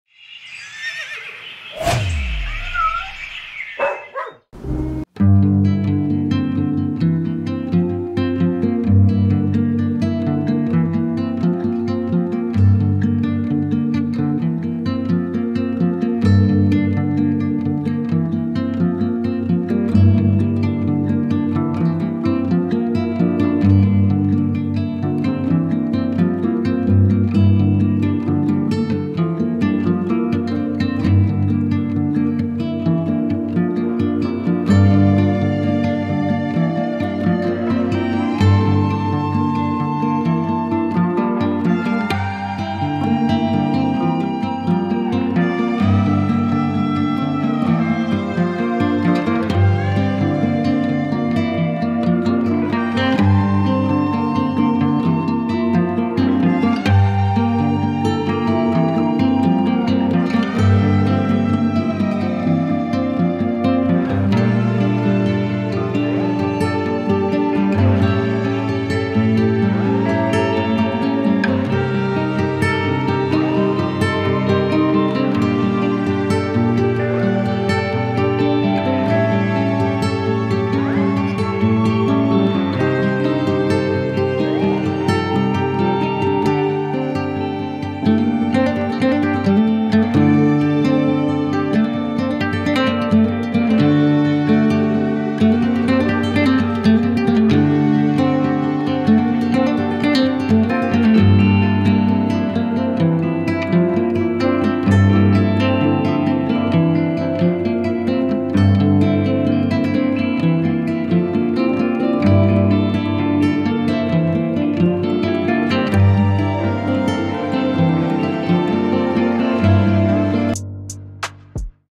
Tumor case | FRIENDICOES SECA
⛔️⚠️Trigger warning ⚠️⛔️ : injuries, animal abuse, blood

After spaying/neutering, the most common surgical procedures that the Friendicoes vets perform are cancer surgeries.

Whether it is the polluted air, or micro plastics in food and water, our environment is becoming increasingly toxic and it affects our animal friends too.

However, animals are surprisingly resilient. A watchful eye is often all they need to survive even in the most terrible situations. Lily is one such lucky girl. She's a street dweller but the person who feeds her noticed an abnormal growth in her abdomen and sent her to Friendicoes Gurgaon hospital.

The growth was a cancerous tumor, but the vets operated upon her immediately. The surgery was successful, and after 10 days' recuperation at Friendicoes, Lily was sent back.

The person feeding her, will now be keeping an eye on her health, and in case we suspect her cancer returning, we'll bring her back for further treatment.

Lily, and countless other animals like her who live on the streets, avail Friendicoes' veterinary services entirely due to public donations. Your support is our life blood. Please keep supporting us!